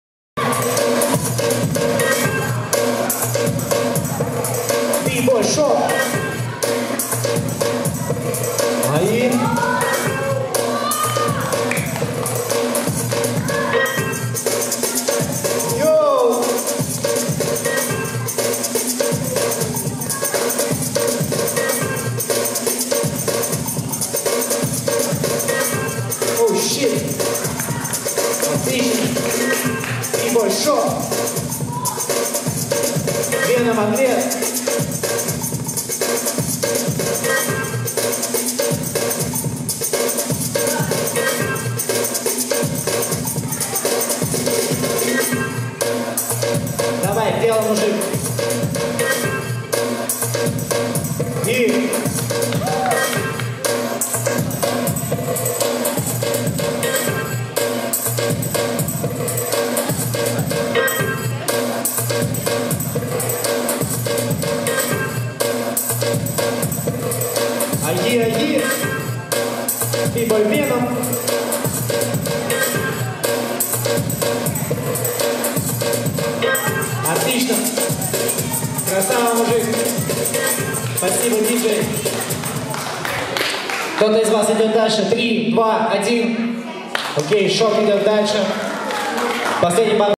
bboy Shock vs Venom - брейкданс чемпионат TurtleFreeze Battle
17 ноября 2019 в г.Раменское прошел брейкданс чемпионат TurtleFreeze Battle.
Категории участников: начинающие, возраст 7-9 лет, 10-14 лет, 18+ мастера.